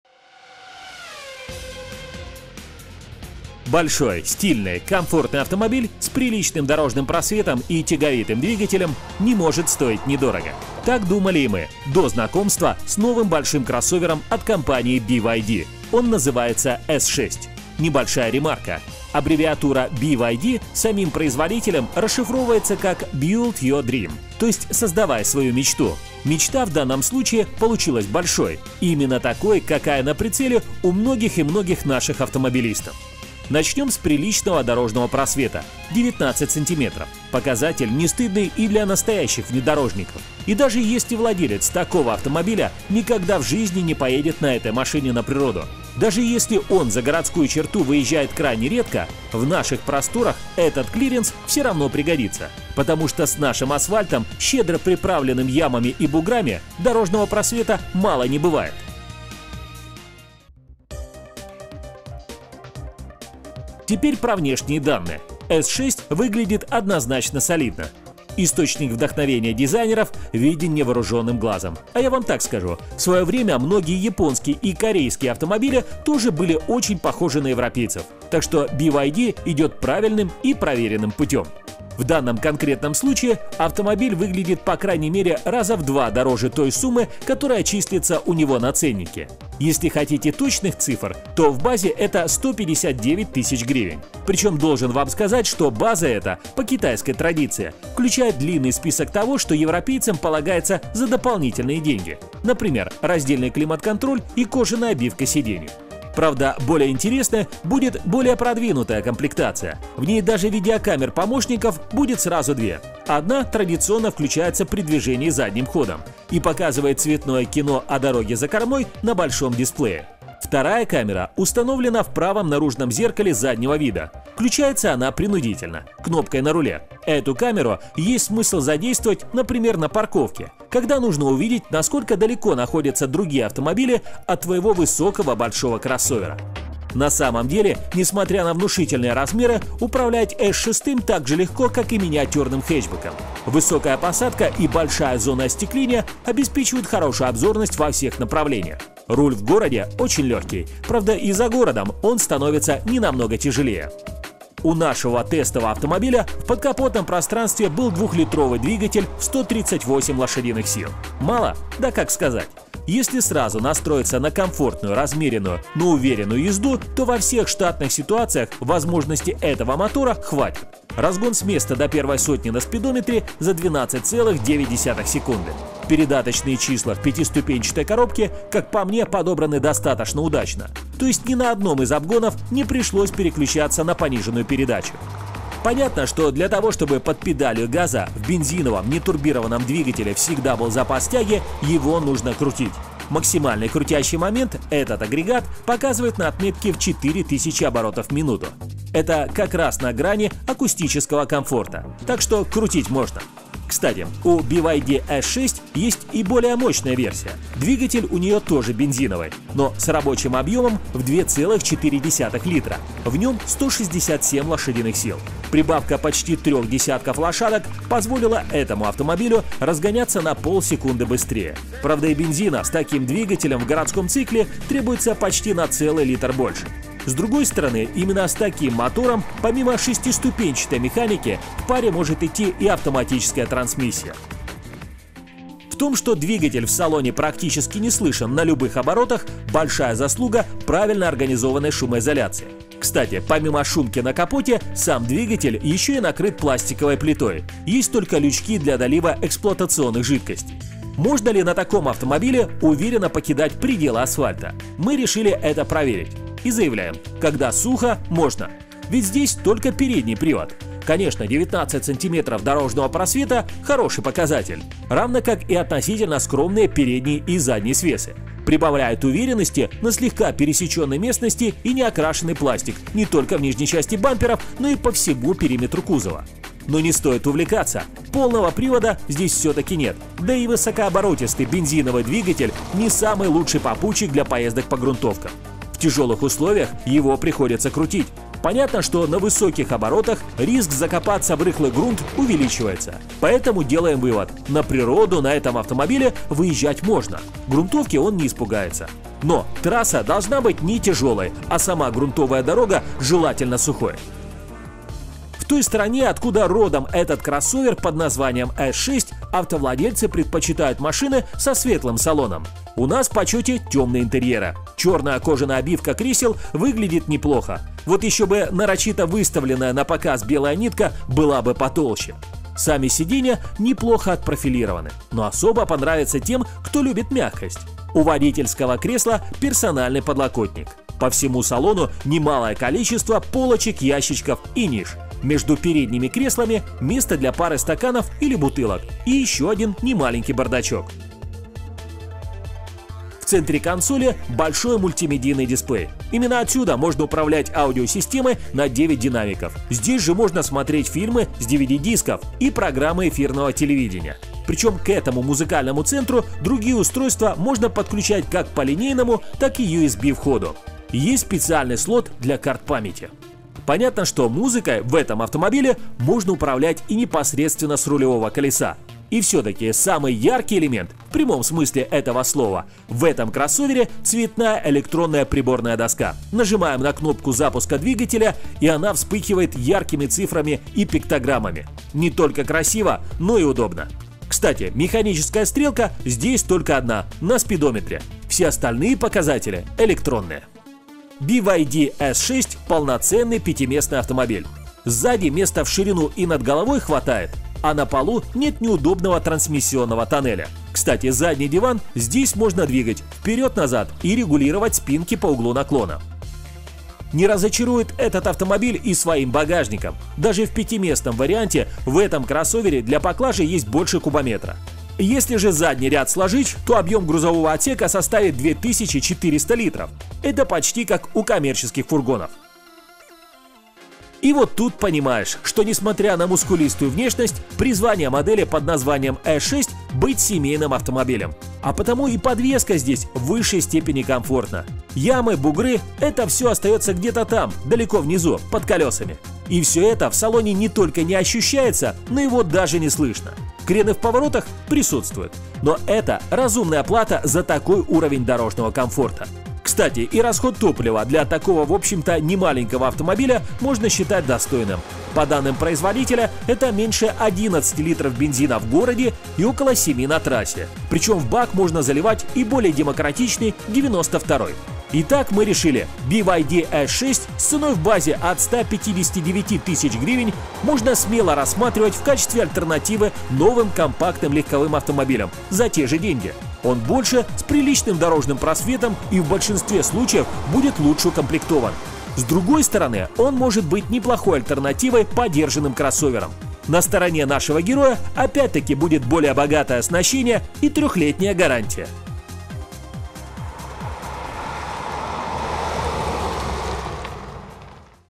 BYD S6, "Кабриолет-ТВ"
Обзор кроссовера BYD S6.
Большой полноразмерный кроссовер по цене компактного легкового автомобиля.
Двигатель 2.0 бензин. Разгон 0-100 км/ч - 12.9 секунды.
Программа для автомобилистов "Кабриолет".